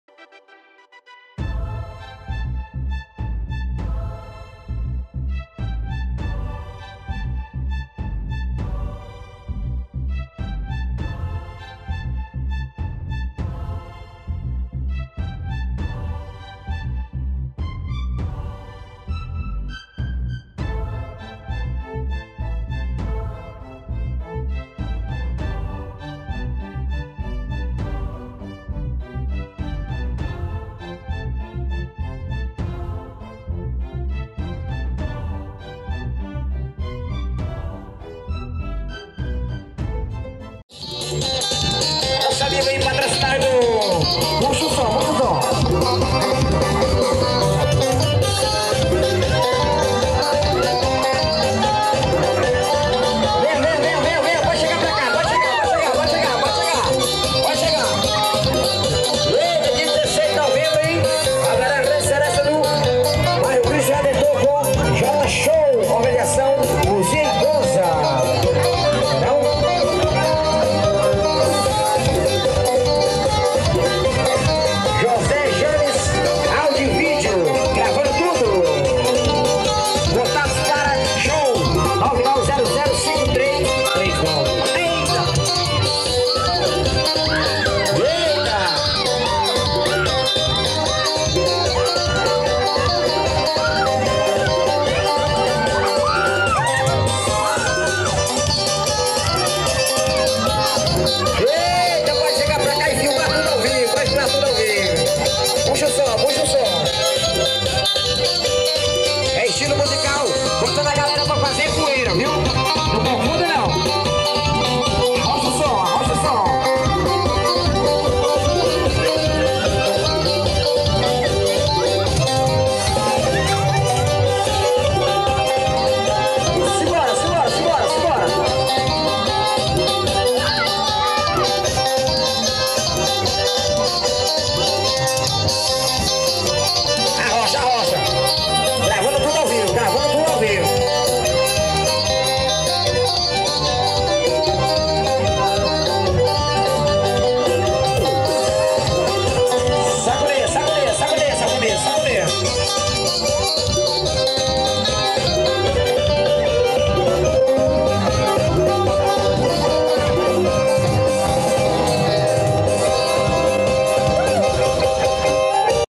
J. JAMES NO FORRÓ AGONIADO [STILLO MUSICAL] CHAPADINHA ESPERANTINA PI - 2019
3ª VAQUEJADA BOI NO MATO NA FAZENDA SÃO SEBASTIÃO CHAPADINHA ESPERANTINA PIAUÍ - 2019
ORG: Branco e Neto vaqueiro
IMAGEM: J. James Áudio e Vídeo